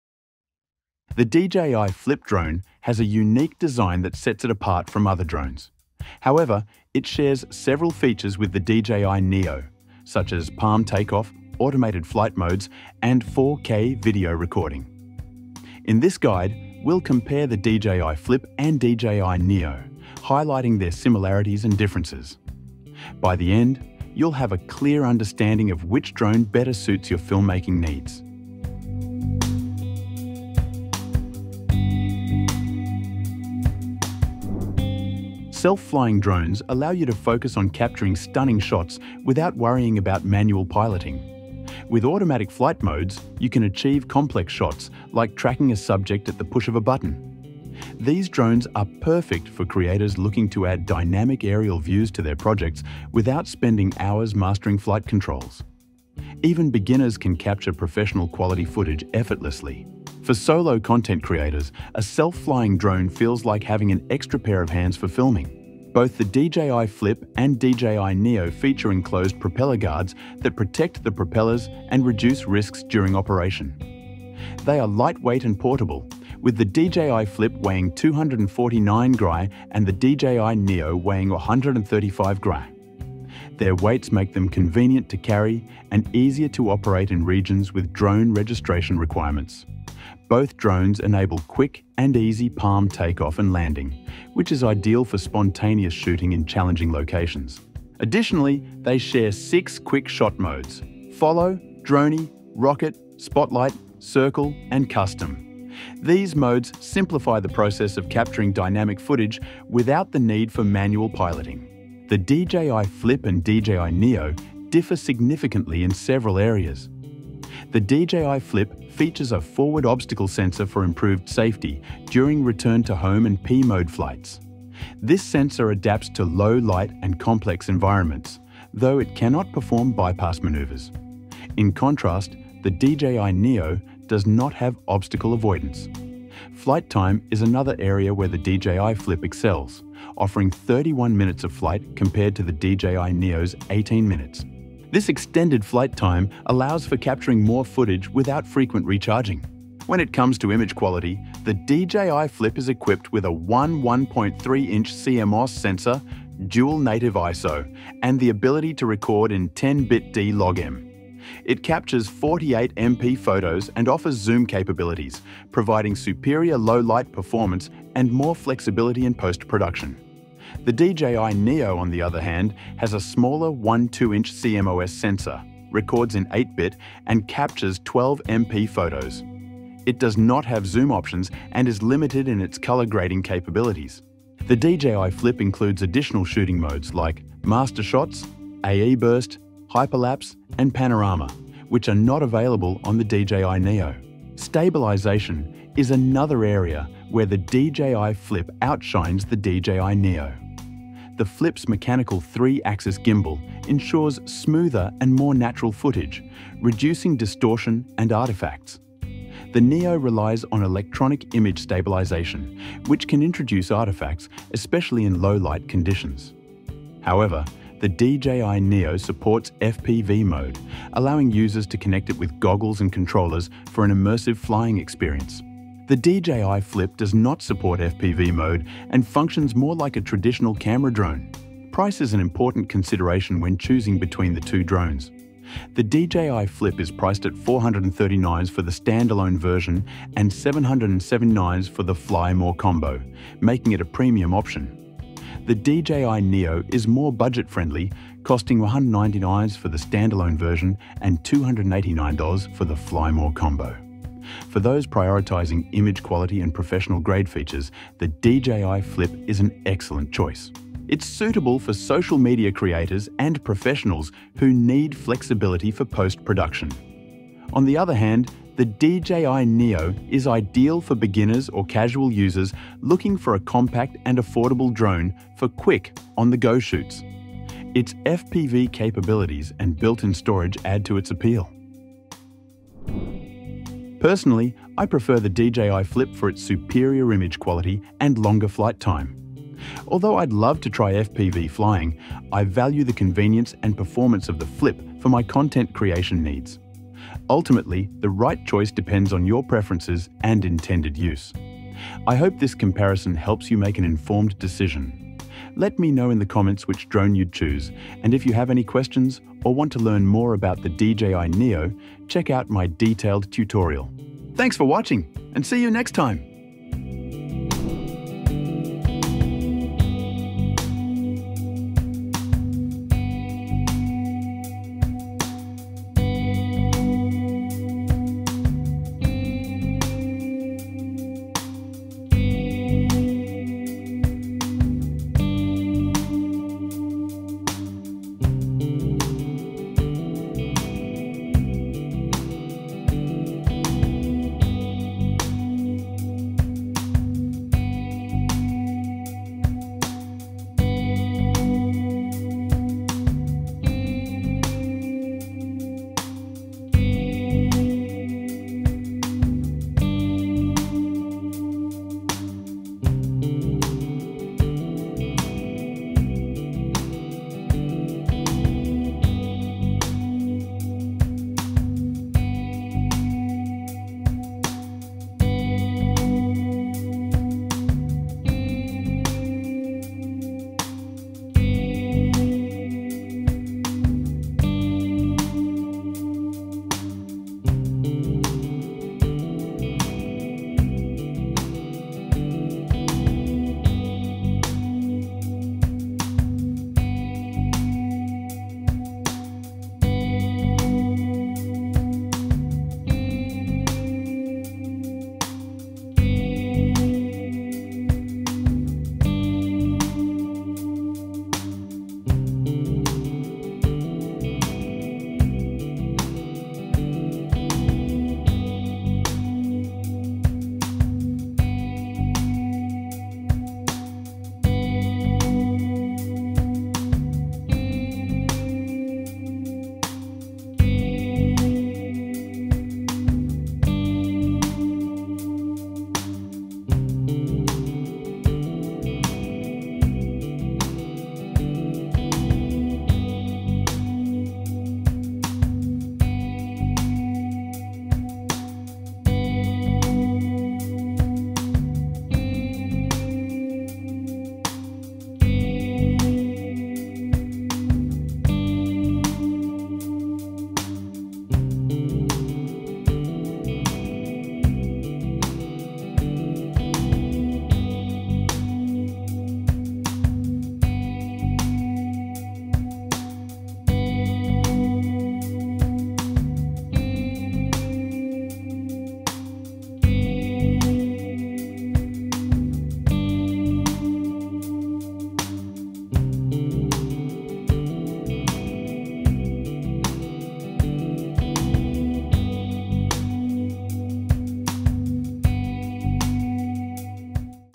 DJI Flip vs DJI Neo - Which Drone Reigns Supreme?
The DJI Flip and DJI Neo drones cater to different user needs while sharing core features like 4K recording, palm takeoff, and automated flight modes. The Flip boasts superior image quality with its 1/1.3-inch sensor, 10-bit D-Log M recording, and 48 MP photos, making it ideal for professionals and content creators. It also offers obstacle avoidance, a longer 31-minute flight time, and a 3-axis gimbal for smoother footage. In contrast, the Neo is lightweight, affordable, and supports FPV mode for immersive flying. With built-in storage and easy portability, it’s perfect for beginners or casual users. Both drones deliver versatility and ease of use, but your choice depends on your priorities—whether it’s professional-grade visuals or convenience and affordability.